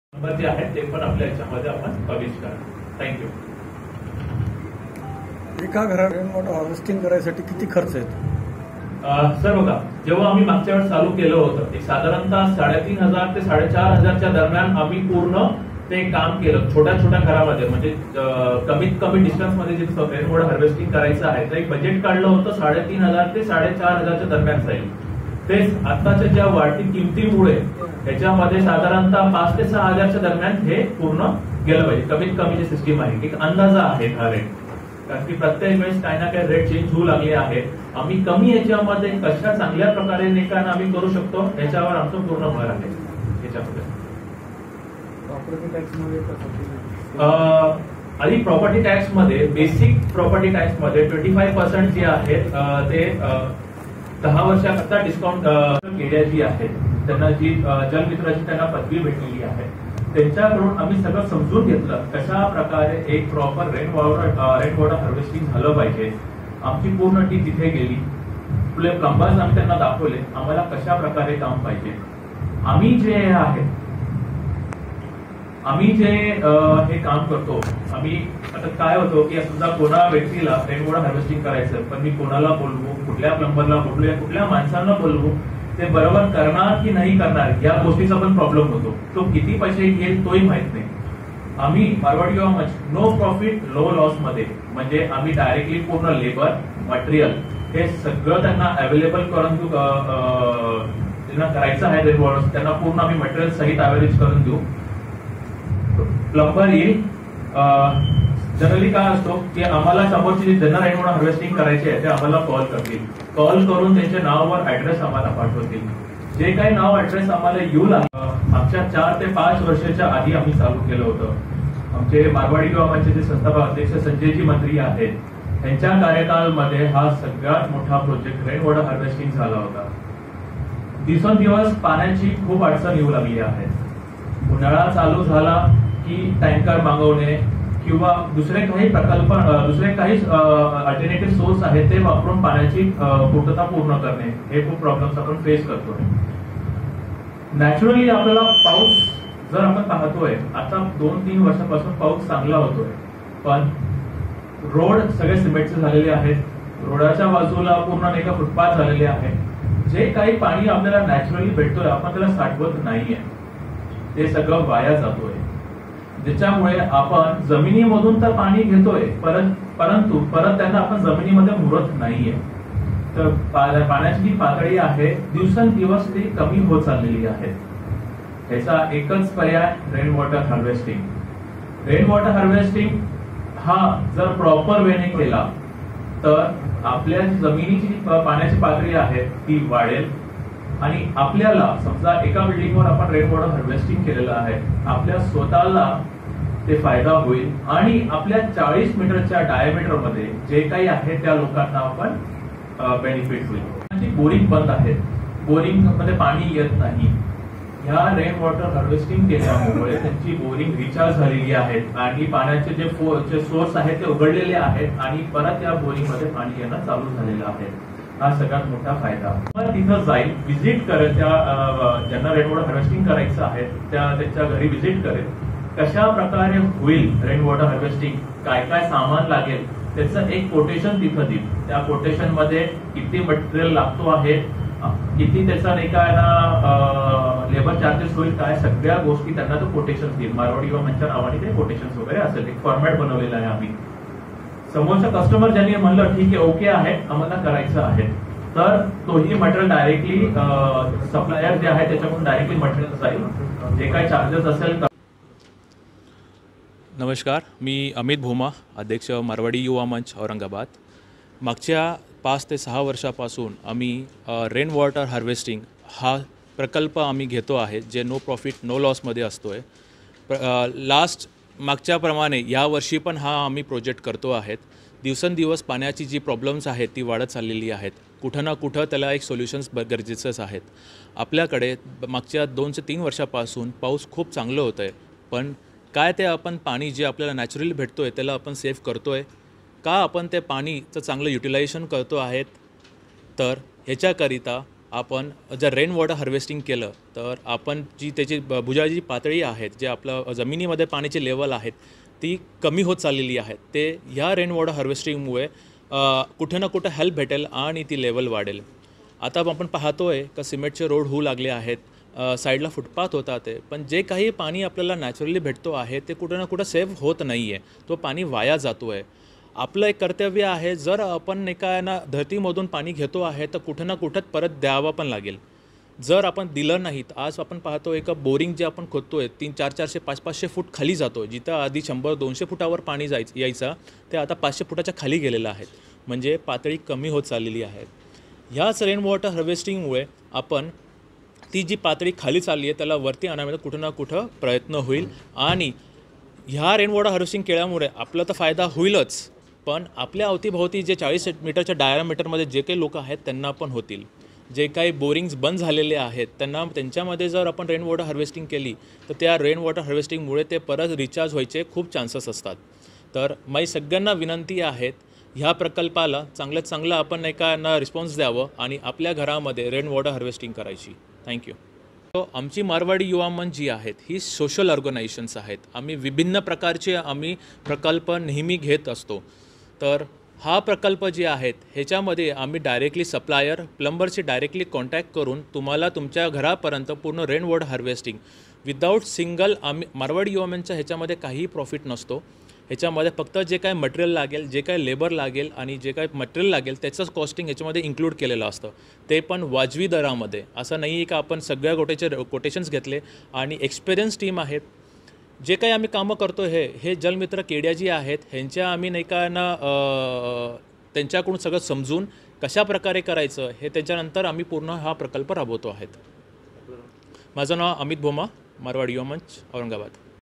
AURANGABAD | शहरातील पाणी प्रश्न सोडवण्यासाठी रेन वॉटर हार्वेस्टिंग मोठ्याप्रमाणात करणार
AURANGABAD | मारवाडी युवा मंच आजची पत्रकार परिषद संपन्न, शहरातील पाणी प्रश्न सोडवण्यासाठी रेन वॉटर हार्वेस्टिंग मोठ्याप्रमाणात करणार

सिडको परिसरातील एका सभाग्रहात मारवाडी युवा मंचच्या ची पत्रकार परिषद संपन्न झाली असून सदरील पत्रकार परिषदेमध्ये शहरातील मोठ्या पाणी प्रश्नासंदर्भात चर्चा करण्यात आली असून यावेळी मारवाडी युवा मंचच्या वतीने औरंगाबाद शहरातील विविध बागांमध्ये रेन वॉटर हार्वेस्टिंग न नफा न तोटा करून देण्यात येणार असल्याची माहिती देण्यात आली आहे. दरवर्षी मारवाडी युवा मंचच्या वतीने शहरात हा उपक्रम करण्यात येतो या वर्षी जास्त प्रमाणात सदरील उपक्रम राबविण्यात येणार असल्याची माहिती मारवाडी युवा मंचाचे अध्यक्ष अमित भोमा यांनी पत्रकार परिषदेमध्ये सदरील माहिती दिली आहे.

सदरील पत्रकार परिषदेला आशिष मेहता निखील खंडेलवाल यांची उपस्थिती होती, शहरातील पाणी पातळी अत्यंत खाली गेली असून यासाठी रेन वॉटर हार्वेस्टिंग करणे गरजेचे आहे यासाठी रेन वॉटर हार्वेस्टिंग कशी करावी व त्याचे फायदे काय यासंदर्भात शहरातील नागरिकांमध्ये मोठ्या प्रमाणात जनजागृतीही करण्यात येणार असल्याची माहिती त्यांनी यावेळी दिली आहे, सदरील पत्रकार परिषद आज रोजी सिडको परिसरात संपन्न झाली आहे.

बाईट- अमित भोमा (जैन युवा मंच अध्यक्ष)